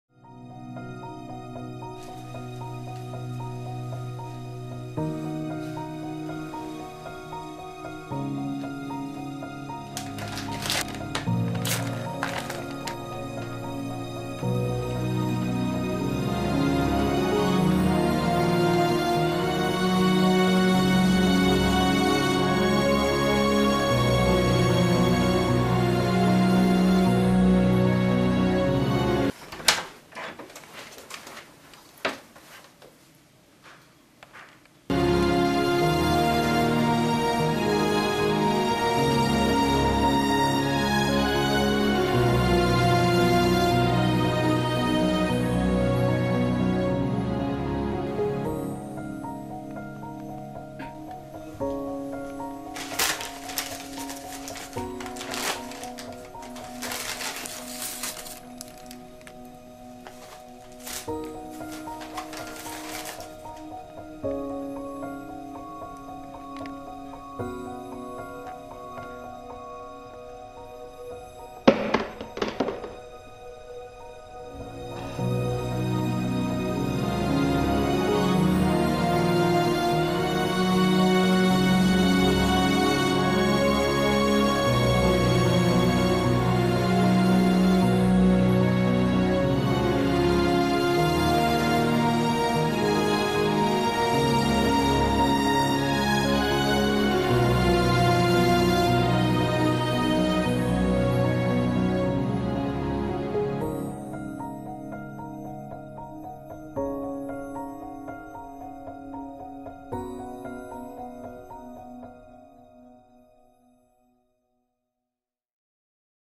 En verden uten poteter
En reklame om hvordan Norge hadde vert uten poteter.